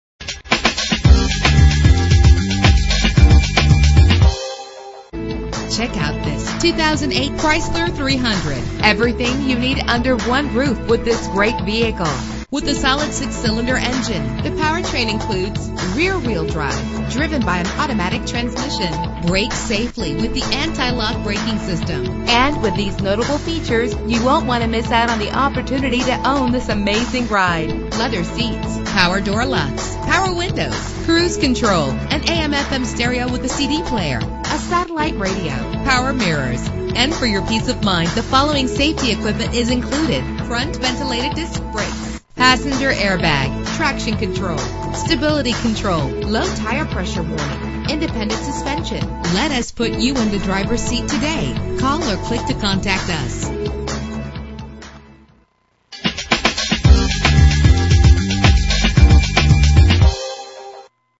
2008 Chrysler 300 - Ocala FL
Year: 2008
Make: Chrysler
Model: 300
Trim: Touring
Engine: 3.5 liter 6 cylinder 24 valve
Transmission: 4-Speed Automatic
Color: Cool Vanilla
Mileage: 45692
Address:
3440 South Pine Avenue